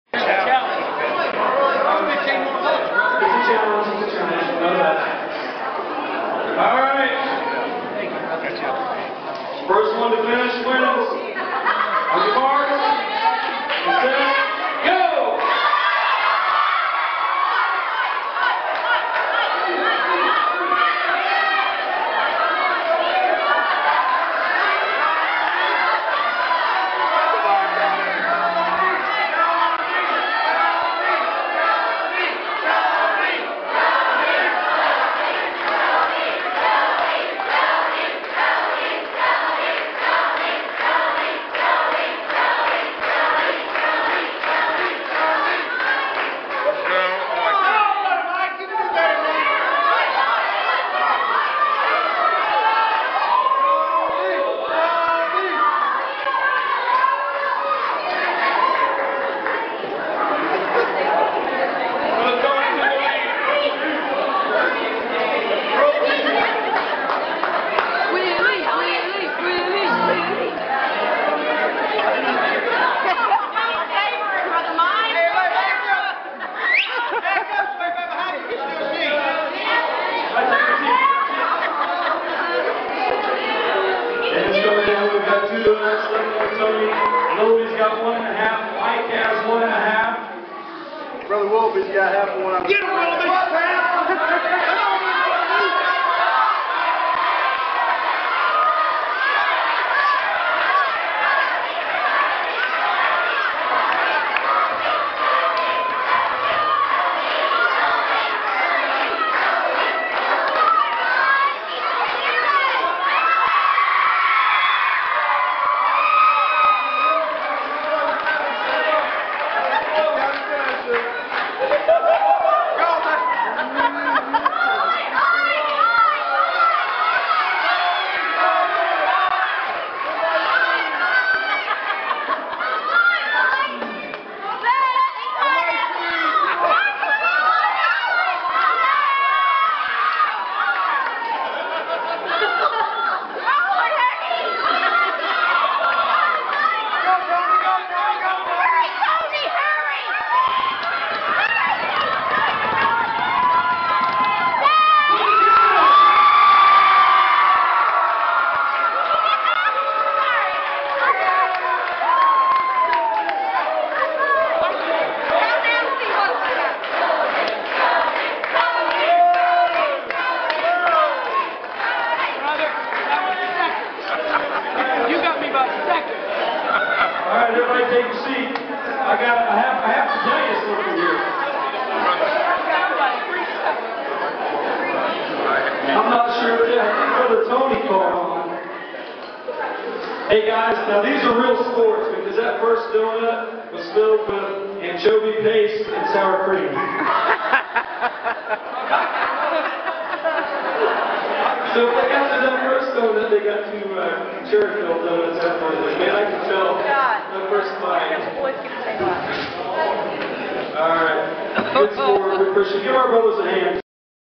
donutcontest
Central Ohio Pentecostal Youth Camp 2009. Mike Harding, Larry Willoughby and Tony Cannon all agree to help us raise a little extra cash to pay the bills last year. These guys were great sports - especially since the first donut had a suprise for the three of them. Rumor has it that Larry switched donuts with Tony - giving Tony 2 defiled donuts. You be the judge.